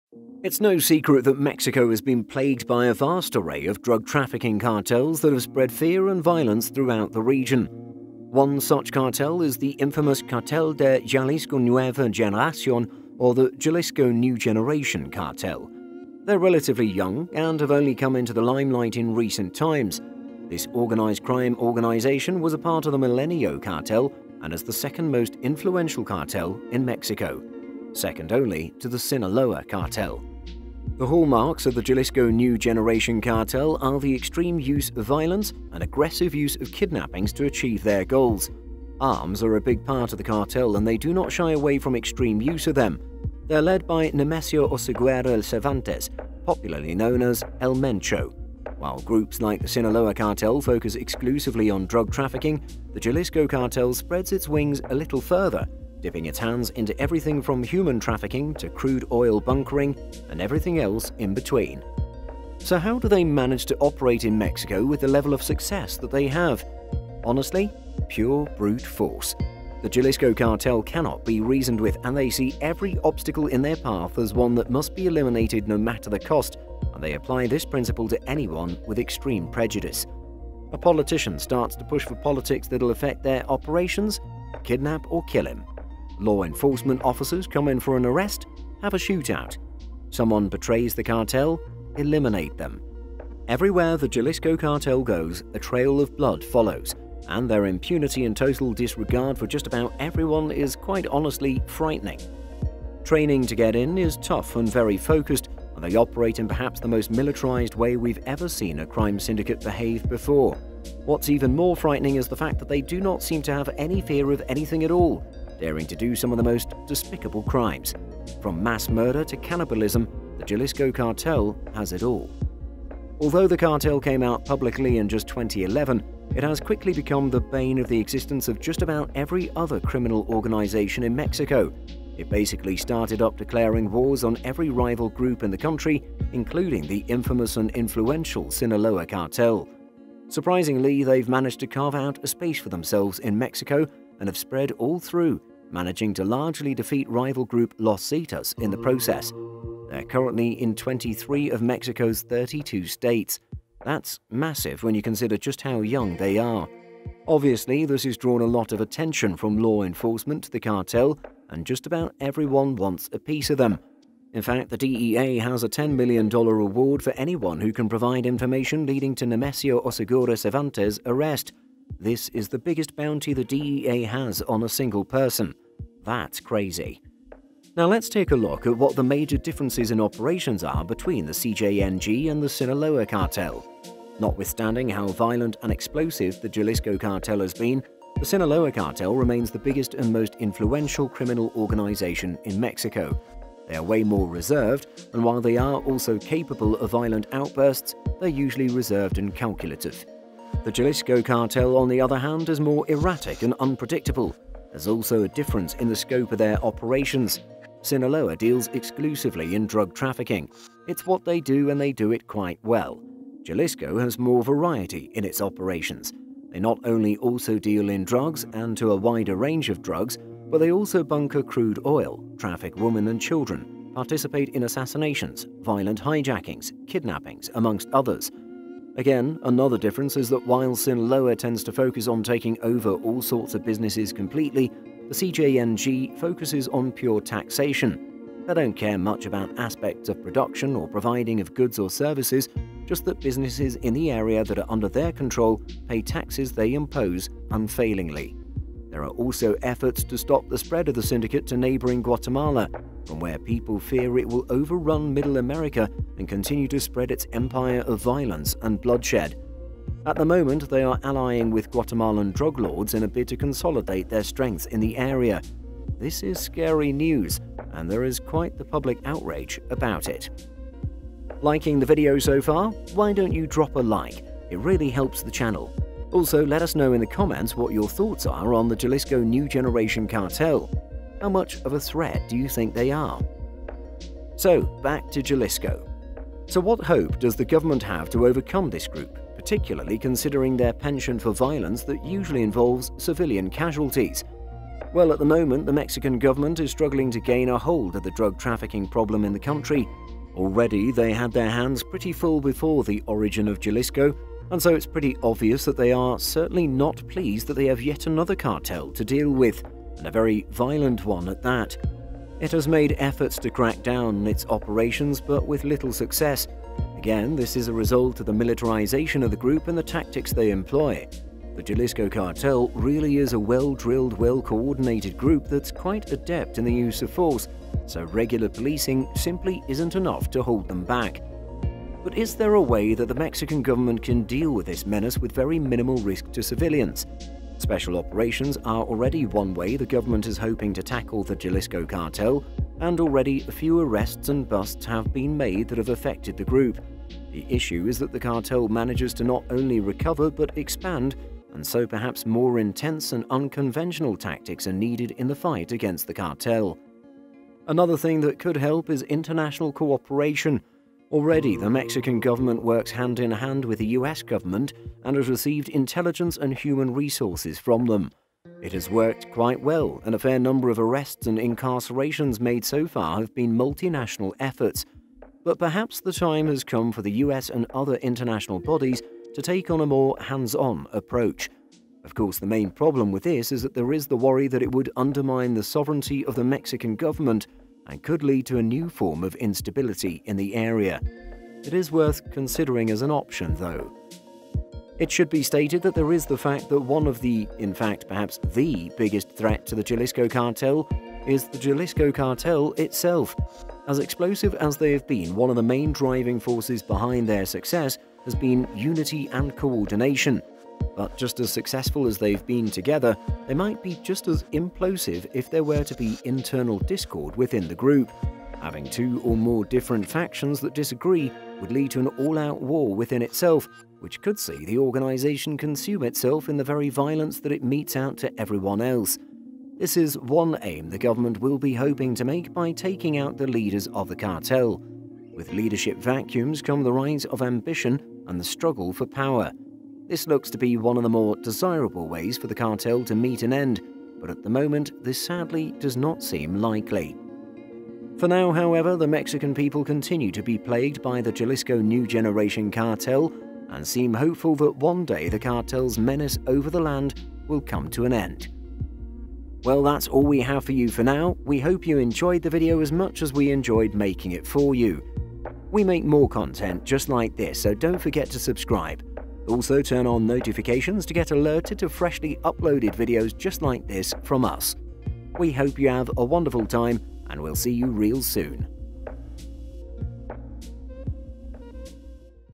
This Is Why You Should NEVER Betray The CJNG
Welcome to Mafia Inside!
In todays video we'll explain how the CJNG cartel works and forcefully operates as Mexico's second biggest criminal organization.

About Mafia Inside
🎨 Written, voiced and produced by Mafia Inside